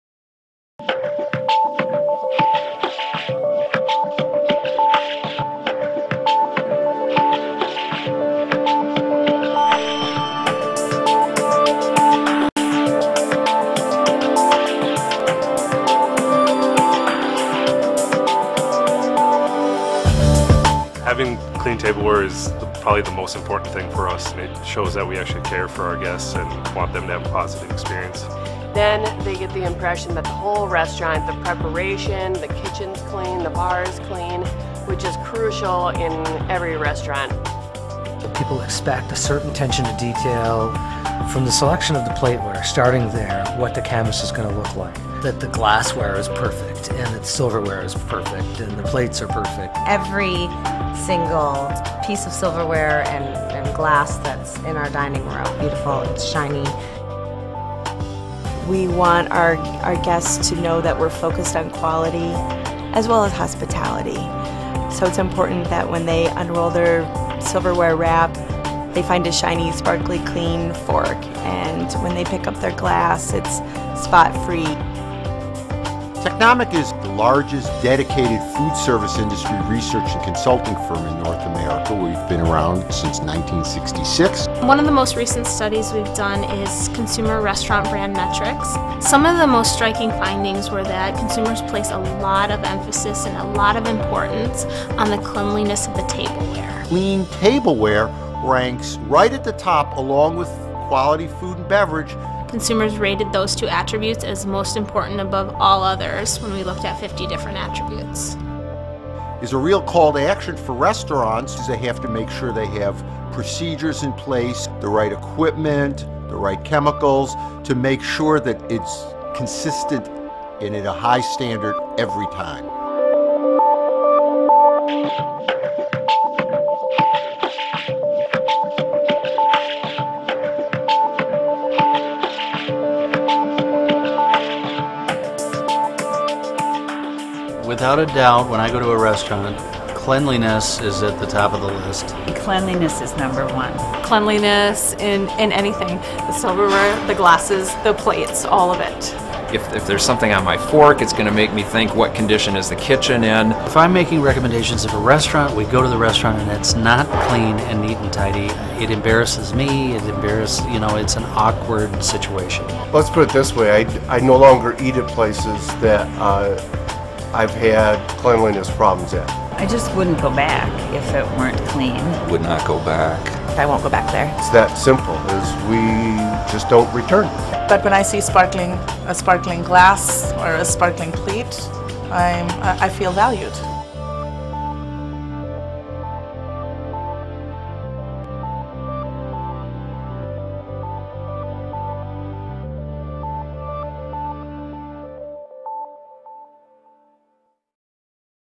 Why Clean Matters
Did you know clean tableware is one of the top factors affecting restaurant guests’ dining experiences? See why sparkling clean glasses and spotless silverware are key to ensuring restaurant diners walk away with positive impressions and keeping coming back for future visits. Watch what real restaurant guests have to say.